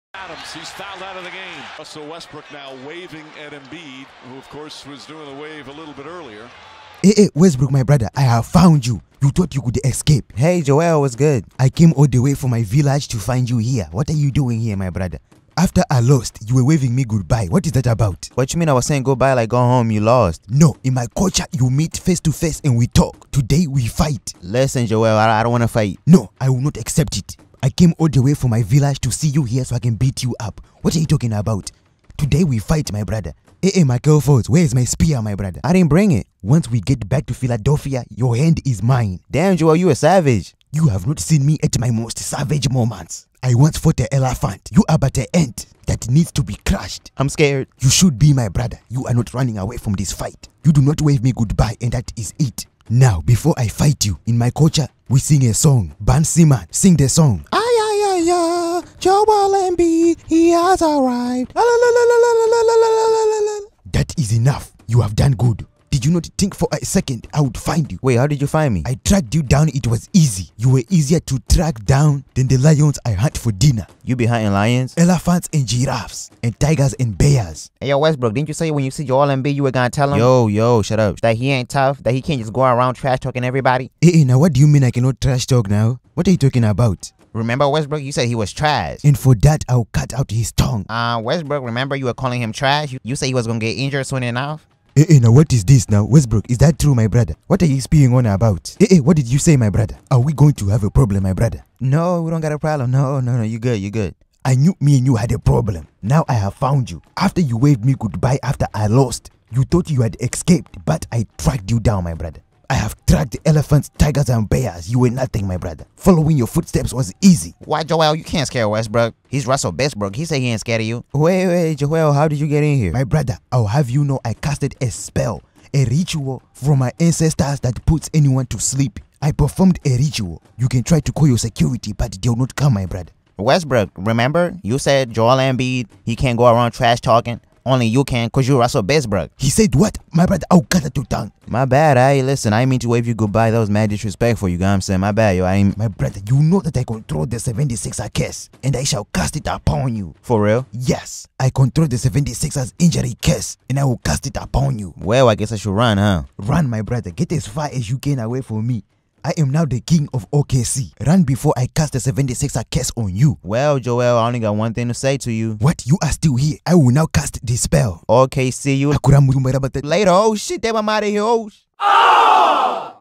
Russell Westbrook RUNS AWAY FROM FIGHT With Joel Embiid!!!
Russell Westbrook RUNS AWAY FROM FIGHT With Joel Embiid (Parody)

This channel is partnered with BBTV NBA Playmakers.